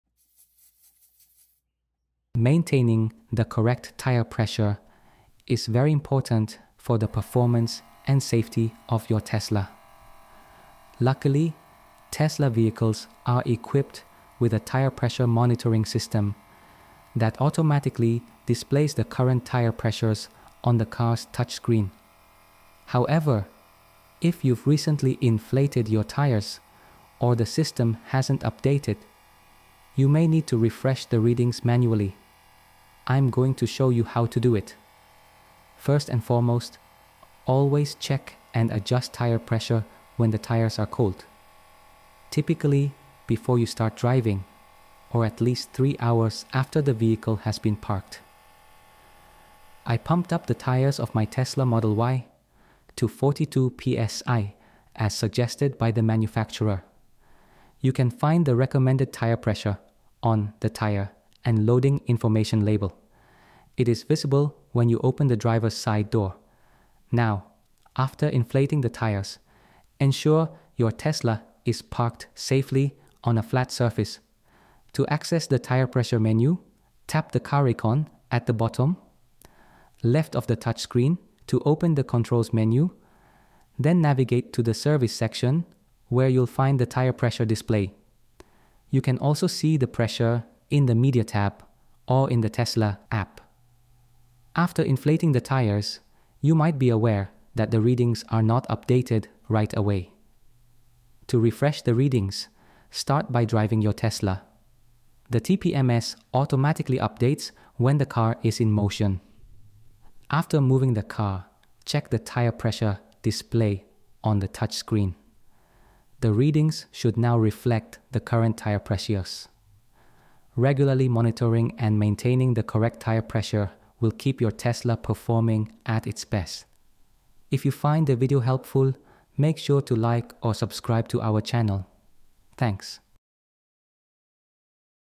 How to Refresh Tire Pressure Readings on Your Tesla After Inflating Tires
The video shows you how to update the tire pressure readings manually after inflating tires on a Tesla.